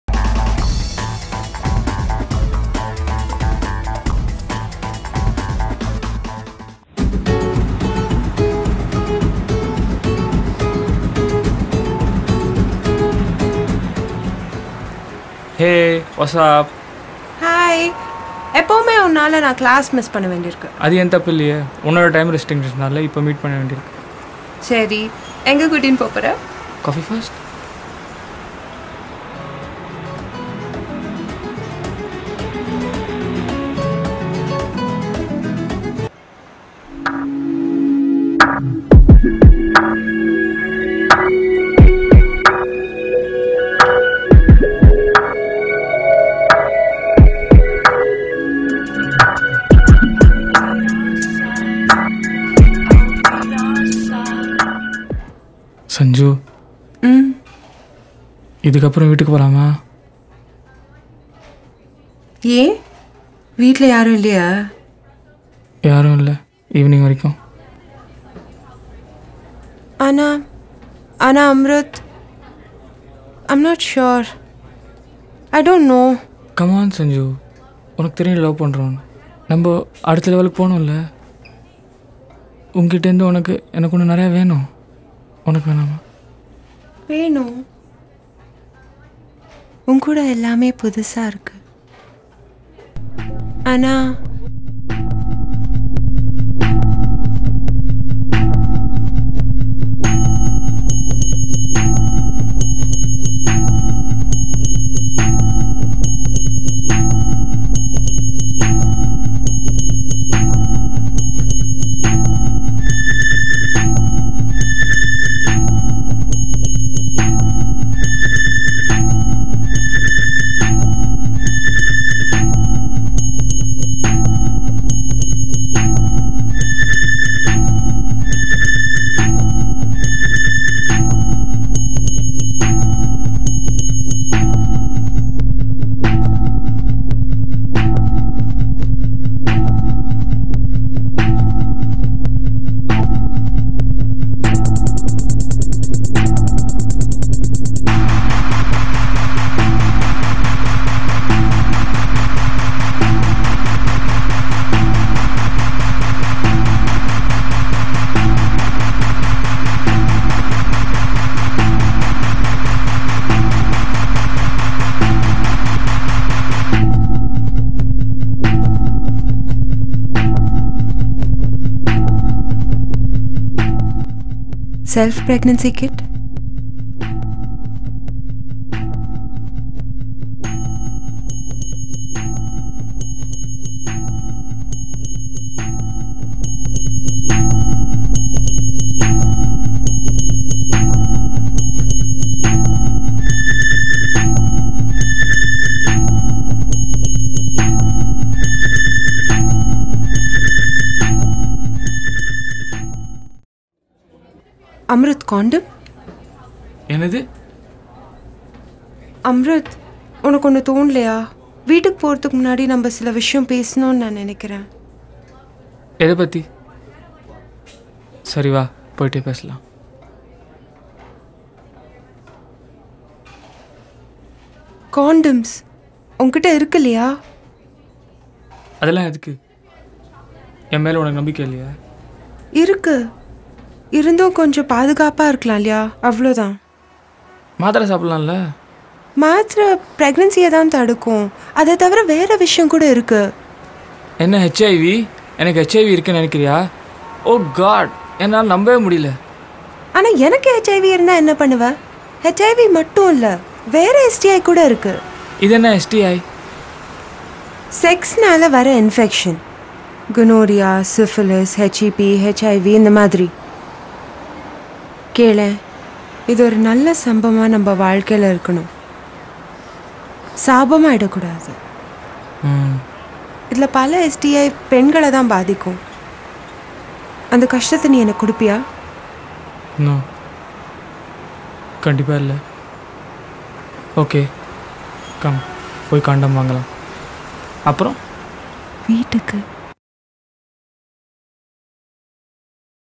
First Time Sex: Young Desi Couple Talk Sex - Tamil
Aditi and Raj are in love, and want to have sex. As a young indian couple, Aditi is concerned, what are the problems she'll face if she just goes over to Raj's house? What about using a condom for pregnancy and STIs? Tune in and find out what they decide!